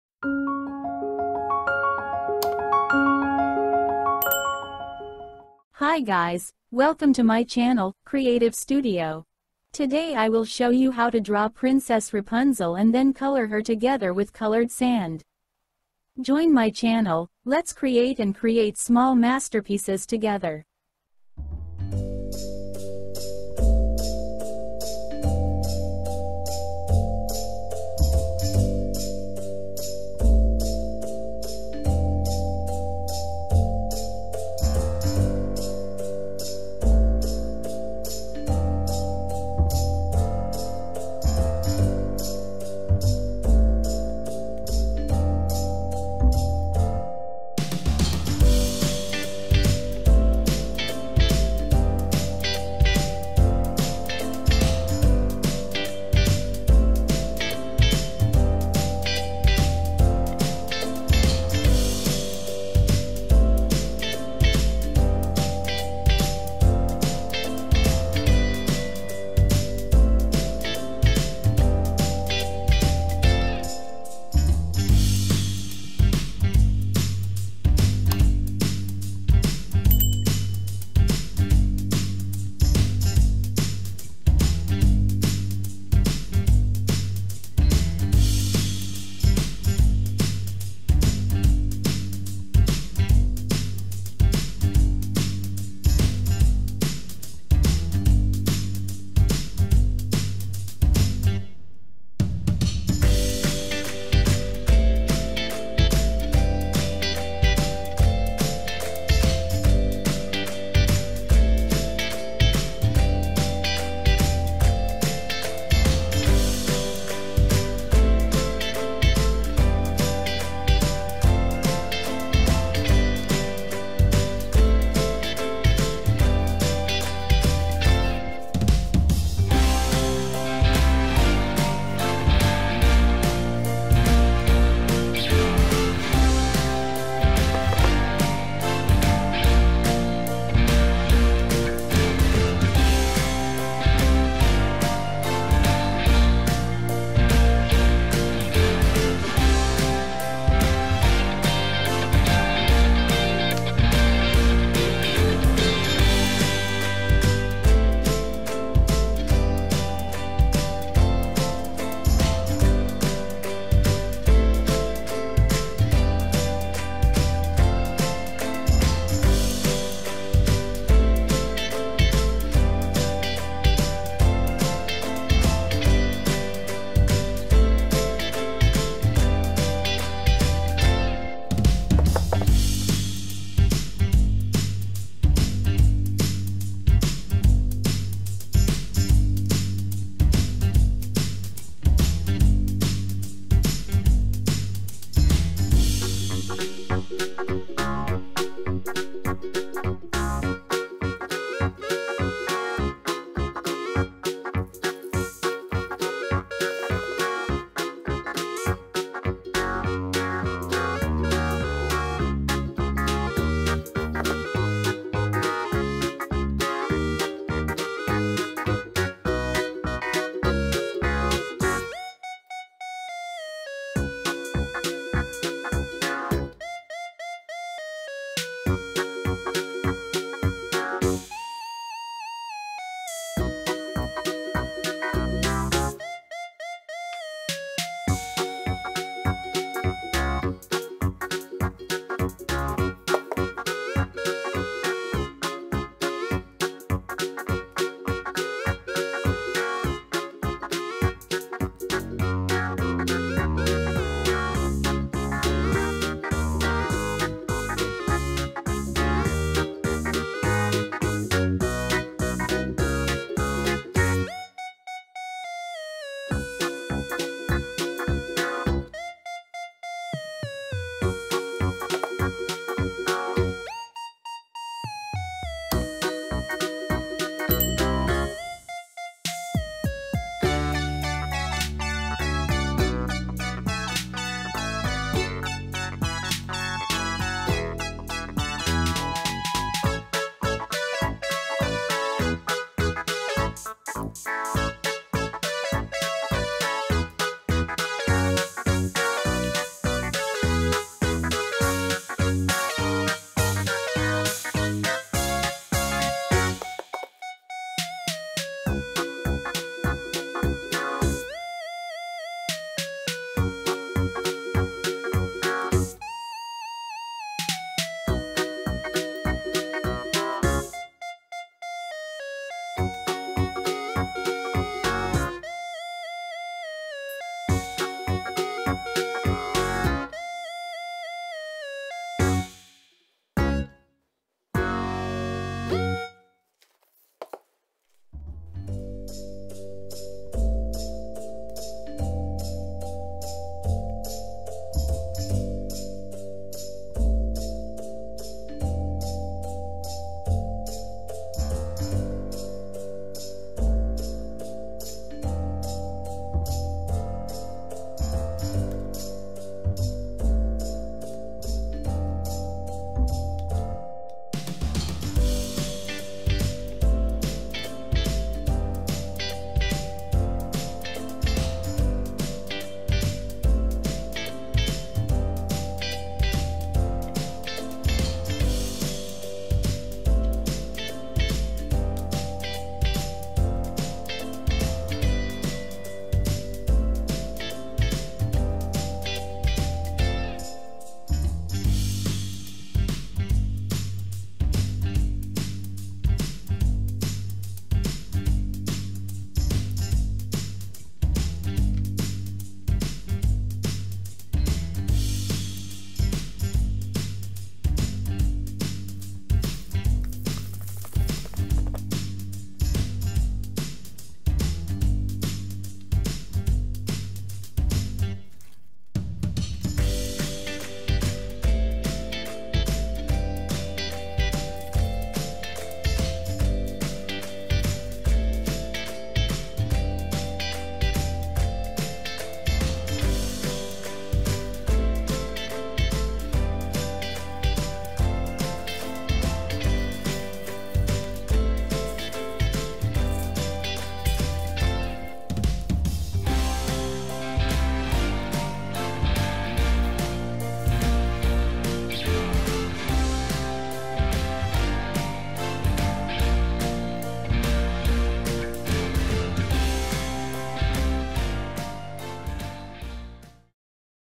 Sand painting, drawing and coloring Rapunzel
Today I will show you how to draw Princess Rapunzel and then color her together with colored sand.